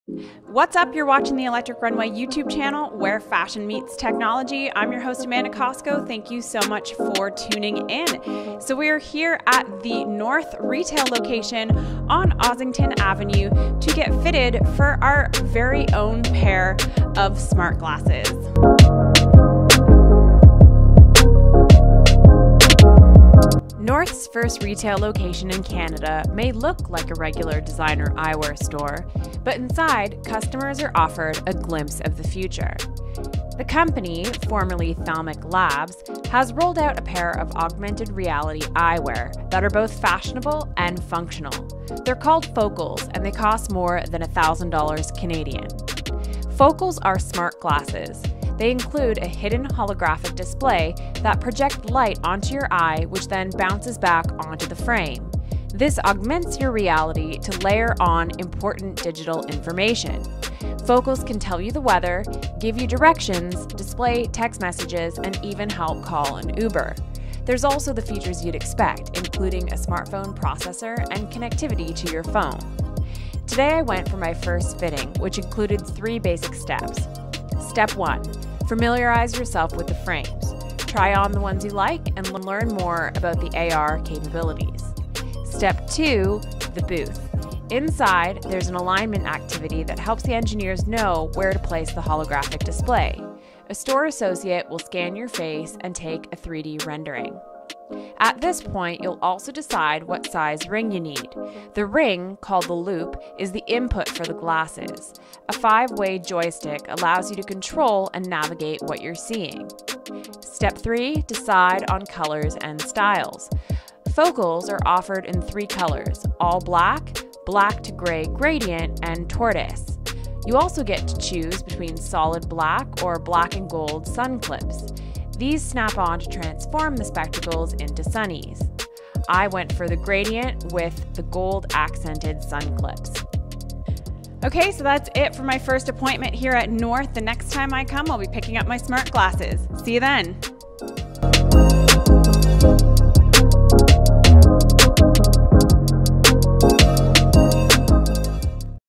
A First Look at Focals by North
North’s first retail location in Canada may look like a regular designer eyewear store, but inside, customers are offered a glimpse of the future. The company, formerly Thalmic Labs, has rolled out a pair of augmented reality eyeglasses that are both fashionable and functional. They’re called Focals, and they cost more than $1000 Canadian. Focals are smart glasses. They include a hidden holographic display that project light onto the frame. This augments your reality to layer on important digital information. Focals can tell you the weather, give you directions, display text messages, and help call you an Uber. There’s also features you’d expect, including a smartphone processor and connectivity to your phone. Come with me as I take you inside North's retail location on Ossington Avenue in Toronto where I'm fitting for my very own pair of smart glasses.

***CORRECTION*** The script in the video inaccurately describes the way the projector inside Focals works. The light is projected onto the frame, not onto your eye. The light passes to the lens first, with the lens reflecting the light back into your eye enabling the user to see the display.